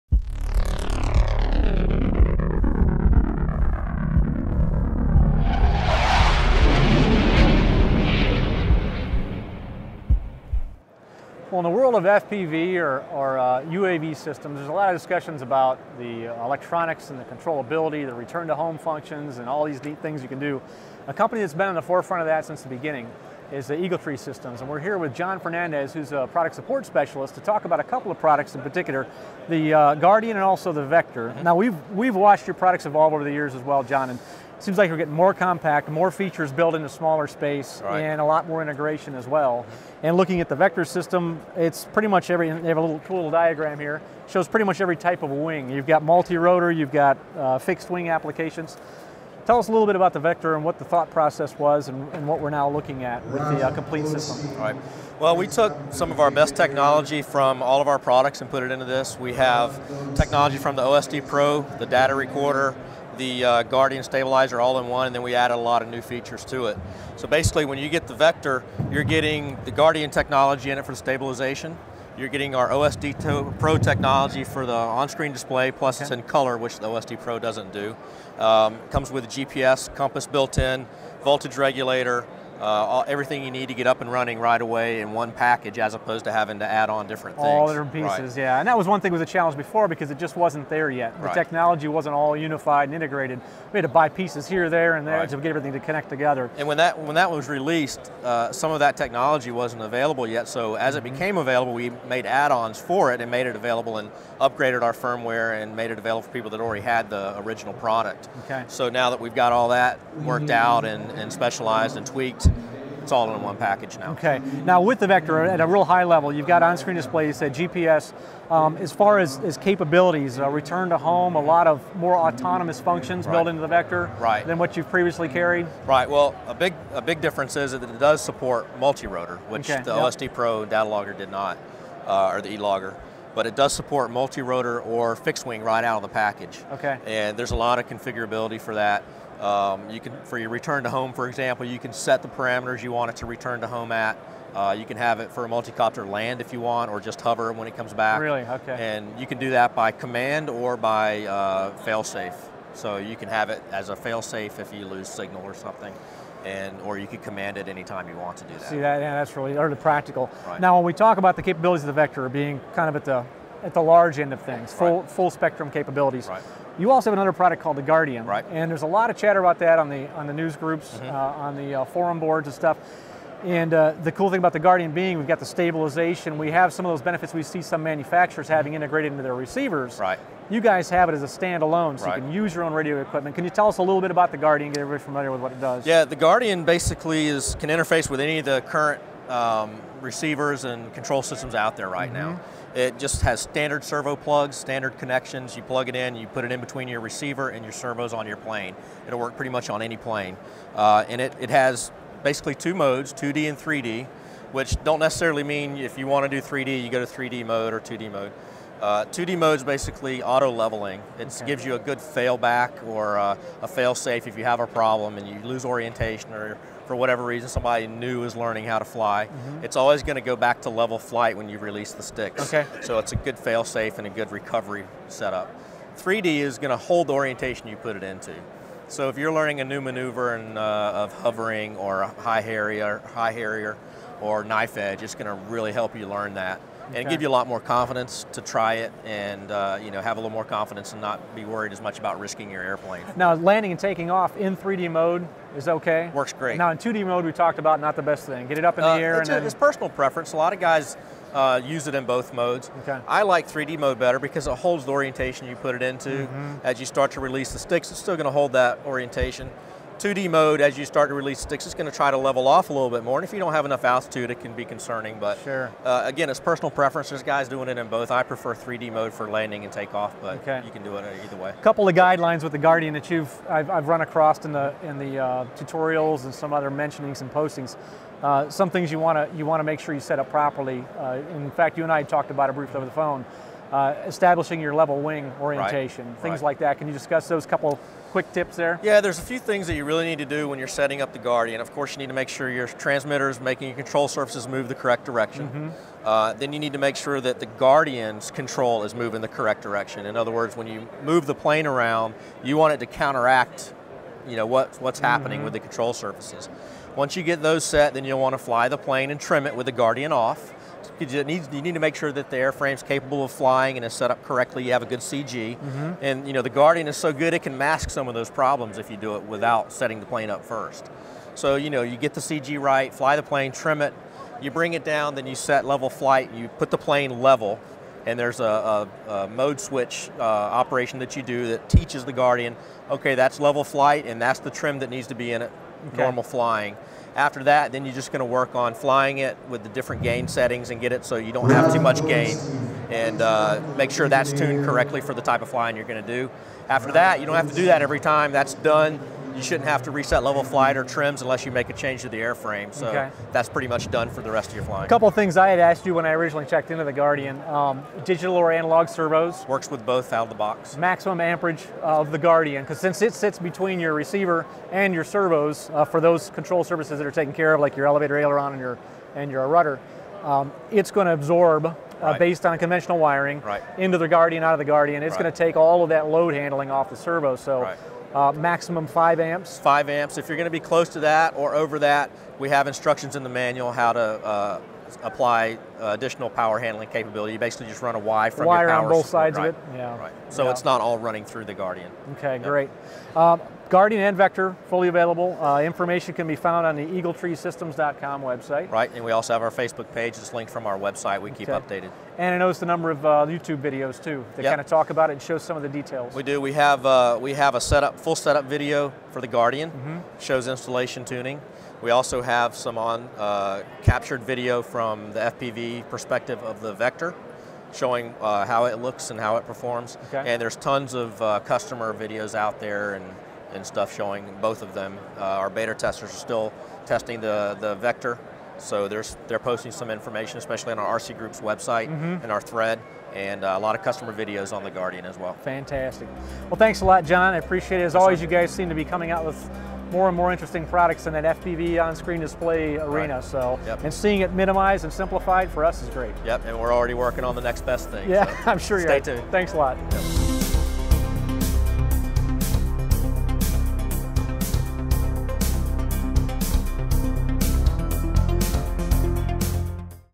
Weak Signals Expo 2014: Eagle Tree Systems Vector OSD+ and Guardian Inertial Stabilizer
To wrap up our tour of the 2014 Toledo Weak Signals show, we stopped by the Eagle Tree Systems booth to get a sneak peek at their new Vector Flight Controller +OSD hardware.  John Fernandez gives us a closer look at what we can expect and all that's packed in this FPV/UAV/OSD marvel.  John also talks about their Guardian 2D/3D inertial stabilizer...and we took one back to the shop for a review!